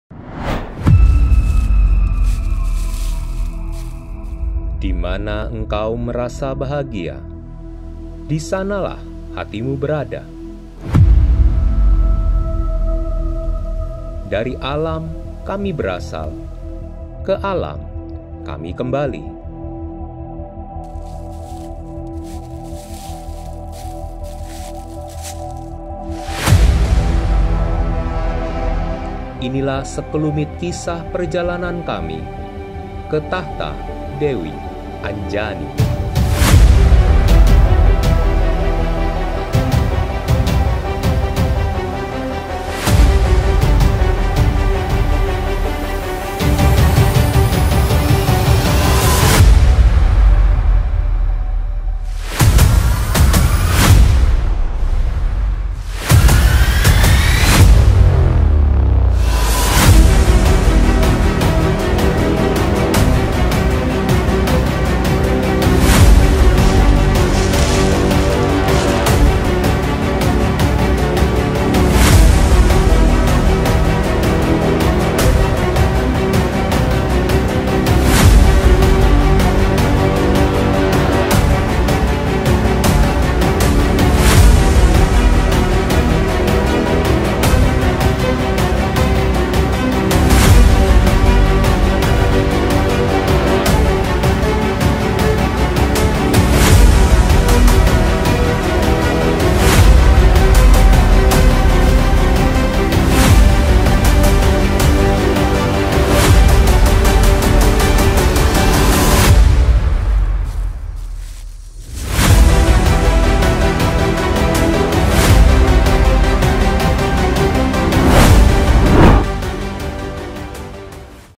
Pendakian Rinjani Via Sembalun to Segara Anak to Senaru (+GPX File)
Gunung Rinjani adalah gunung yang berlokasi di Pulau Lombok, Nusa Tenggara Barat. Gunung yang merupakan gunung berapi tertinggi kedua atau gunung tertinggi ketiga di Indonesia dengan ketinggian 3.726 mdpl
Di sebelah barat kerucut Rinjani terdapat kaldera dengan luas sekitar 3.500 m × 4.800 m, memanjang kearah timur dan barat. Di kaldera ini terdapat Segara Anak (segara= laut, danau) seluas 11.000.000 m persegi dengan kedalaman 230 m. Air yang mengalir dari danau ini membentuk air terjun yang sangat indah, mengalir melewati jurang yang curam.
Di sisi timur kaldera terdapat Gunung Baru Jari yang memiliki kawah berukuran 170m×200 m dengan ketinggian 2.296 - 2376 mdpl.